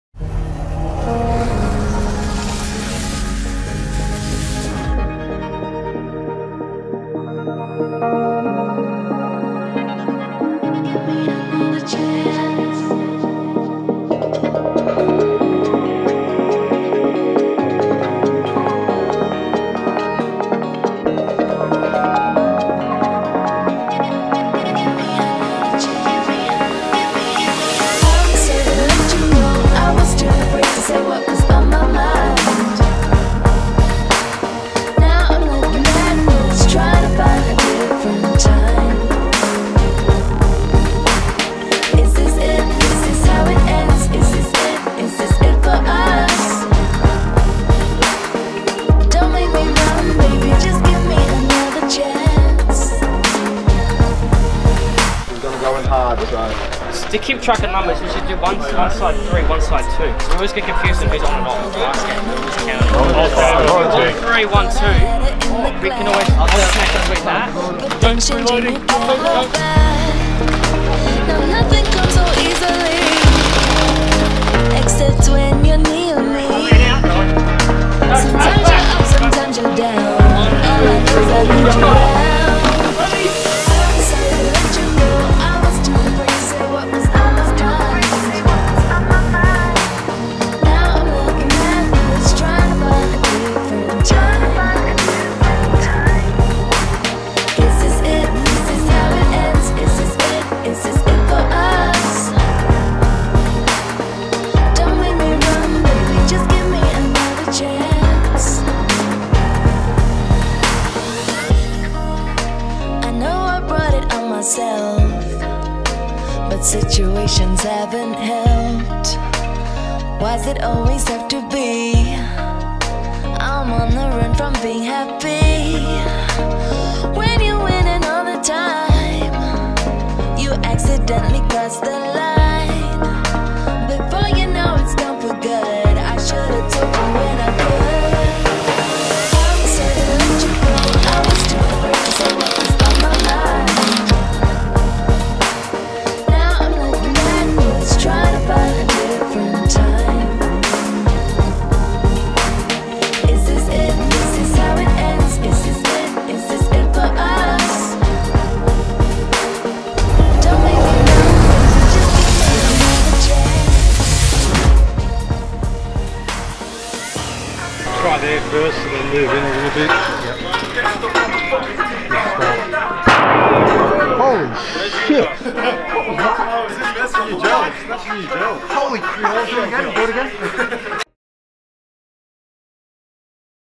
League Day June 2019
Footage of Sloperators and other competitors at the Guerilla Blasters CQB League Speedball!